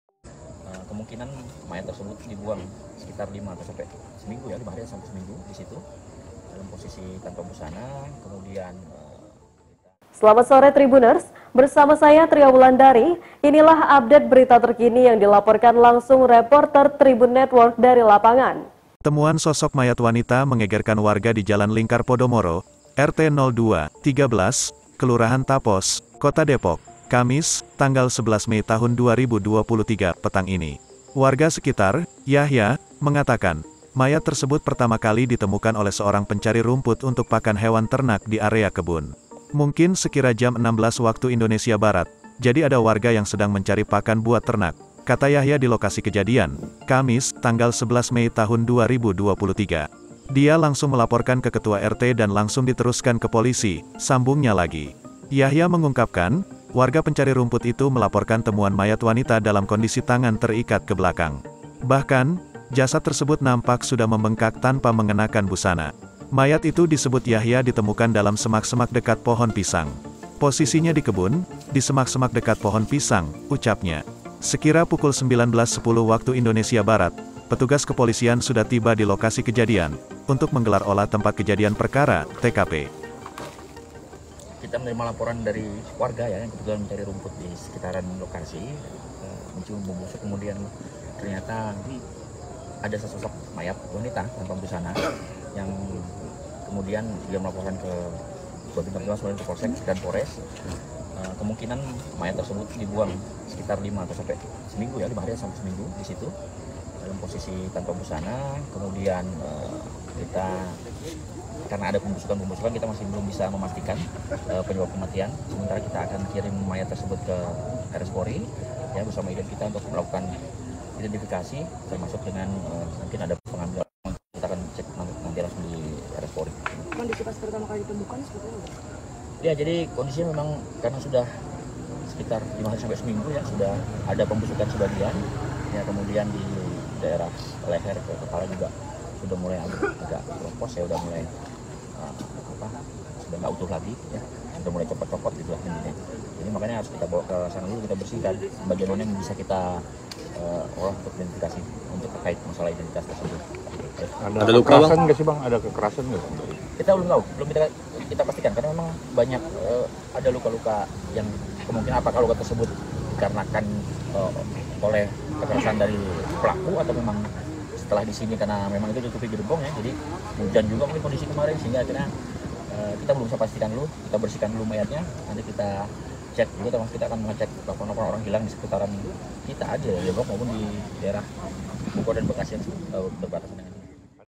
Polisi Sebut Jasad Wanita Tanpa Busana yang Ditemukan, Diduga Tewas Seminggu yang Lalu
Temuan sosok mayat wanita mengegerkan warga di Jalan Lingkar Podomoro, RT 02/13, Kelurahan Tapos, Kota Depok, Kamis (11/5/2023) petang ini.

Warga sekitar, Yahya, mengatakan, mayat tersebut pertama kali ditemukan oleh seorang pencari rumput untuk pakan hewan ternak di area kebun.

"Mungkin sekira jam 16.00 WIB, jadi ada warga yang sedang mencari pakan buat ternak," kata Yahya di lokasi kejadian, Kamis (11/5/2023).

(tyo)